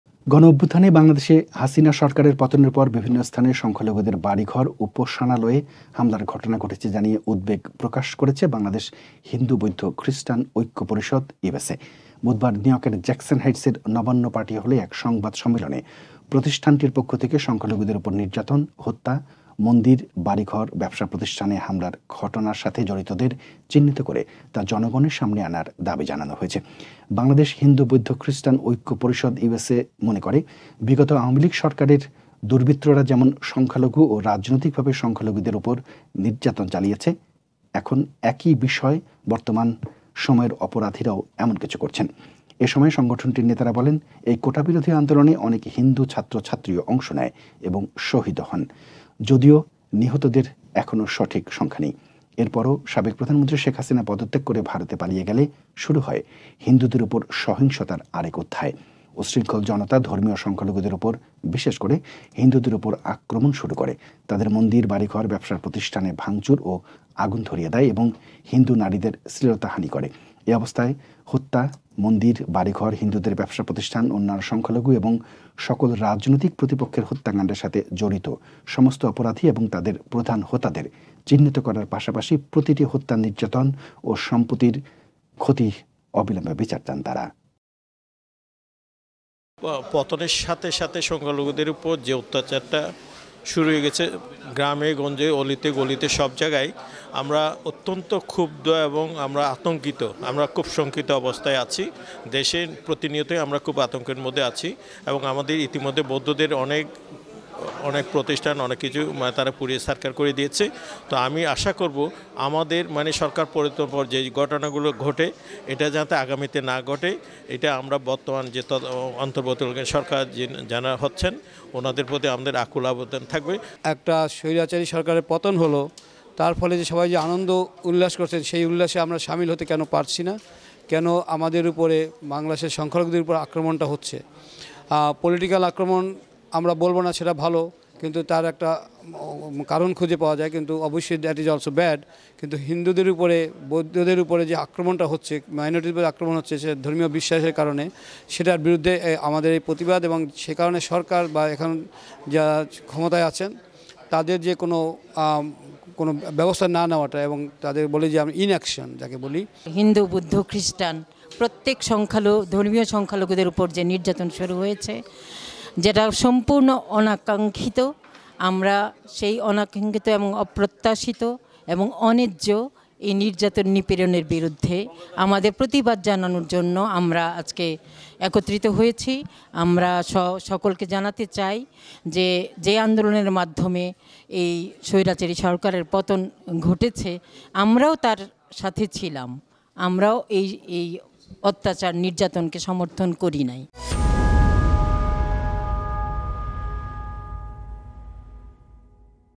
উদ্বিগ্ন বাংলাদেশ হিন্দু বৌদ্ধ খ্রিষ্টান ঐক্য পরিষদ | Hindu Buddhist Christian Unity Parishad USA
গণ-অভ্যুত্থানে বাংলাদেশে হাসিনা সরকারের পতনের পর বিভিন্ন স্থানে সংখ্যালঘুদের বাড়িঘর, উপাসনালয়ে হামলার ঘটনা ঘটেছে জানিয়ে উদ্বেগ প্রকাশ করেছে বাংলাদেশ হিন্দ বৌদ্ধ খ্রিষ্টান ঐক্য পরিষদ-ইউএসএ।  বুধবার নিউইয়র্কের জ্যাকসন হাইটসের নবান্ন পার্টি হলে এক সংবাদ সম্মেলনে প্রতিষ্ঠানটির পক্ষ থেকে  সংখ্যালঘুদের ওপর নির্যাতন, হত্যা, মন্দির -বাড়িঘর- ব্যবসা প্রতিষ্ঠানে হামলার ঘটনার সাথে জড়িতদের চিহ্নিত করে তা জনগণের সামনে আনার দাবি জানানো হয়েছে।

Attacks on minority homes, places of worship, worried Bangladesh Hindu Buddhist Christian Unity Parishad-USA.